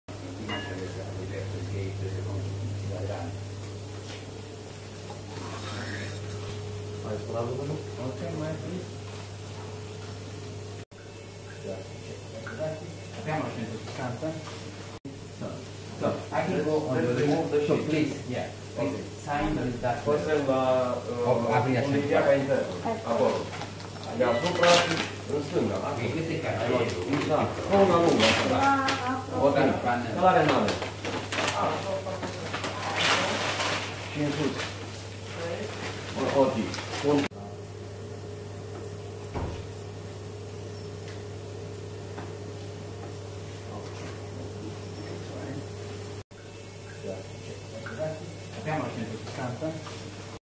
Premiera nationala la Spitalul de Cardiologie Clinicco Brasov
Echipa de medici cariologi interventionali, cu sprijinul colegilor chirurgi cardiovasculari si anestezisti, au efectuat o interventie minim invaziva de endoprotezare a unui anevrism de aorta abdominala juxtarenal, cu ajutorul unei endoproteze de ultima generatie, folosita pentru prima oara in Romania. Pacientul, in varsta de 65 de ani, prezenta boli asociate multiple, severe, iar in urma cu mai multi ani suferise o interventie chirurgicala pe cord deschis.